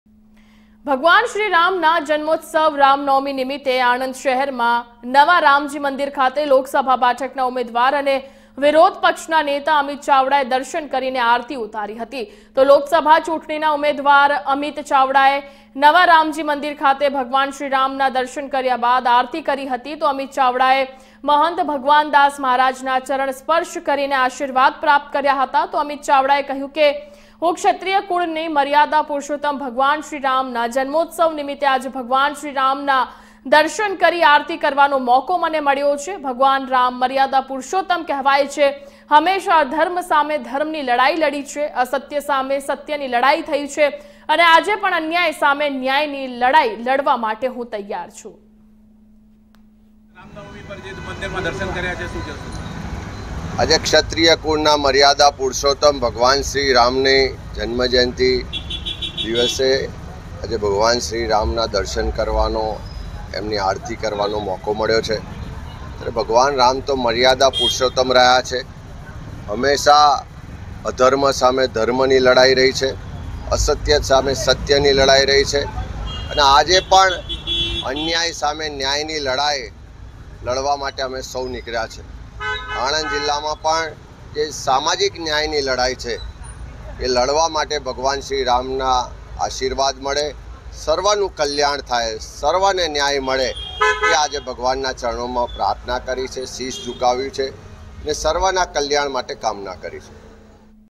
અમિત ચાવડાએ સર્વના કલ્યાણ માટે કામના કરી - India News Gujarat
ભગવાન શ્રી રામના જન્મોત્સવ રામ નવમી નિમિત્તે આજે આણંદ શહેરમાં નવા રામજી મંદિર ખાતે લોકસભા બેઠકના ઉમેદવાર અને વિરોધ પક્ષના નેતા અમિત ચાવડાએ દર્શન કરી  આરતી ઉતારી હતી.લોકસભા ચૂંટણીનાં ઉમેદવાર અમિત ચાવડાએ નવા. રામજી મંદિર ખાતે ભગવાન શ્રી રામના દર્શન કર્યા બાદ આરતી ઉતારી હતી, અમિત ચાવડાએ મહંત ભગવાનદાસ મહારાજનાં ચરણ સ્પર્શ કરી આશીર્વાદ પ્રાપ્ત કર્યા હતા.

On the occasion of Lord Shri Ram's birth festival Ram Navami, the Lok Sabha seat candidate and leader of the opposition Amit Chawda paid a darshan and offered aarti at New Ramji temple in Anand city today. Lok Sabha election candidate Amit Chawda.